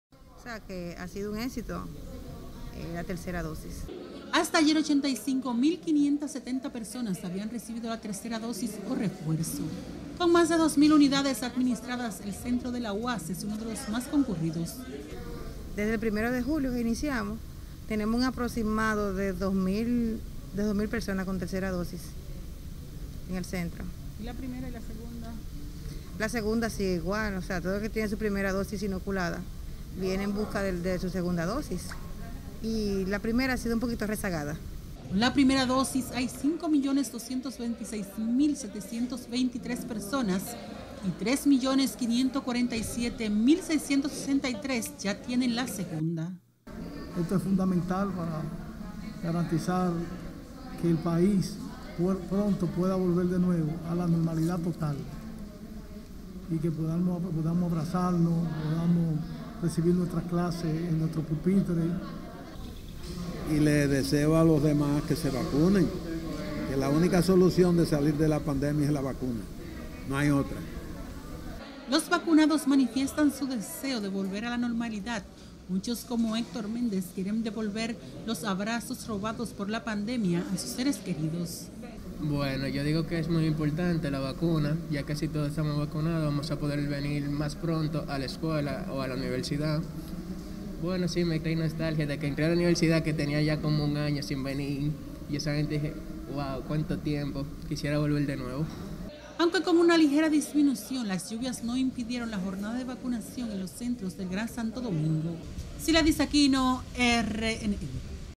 RD llega a 3.5 MM vacunados con ambas dosis
La República Dominicana llegó a las 3,547,663 personas que han completado el esquema de vacunación, con la aplicación de 8,859,966 dosis.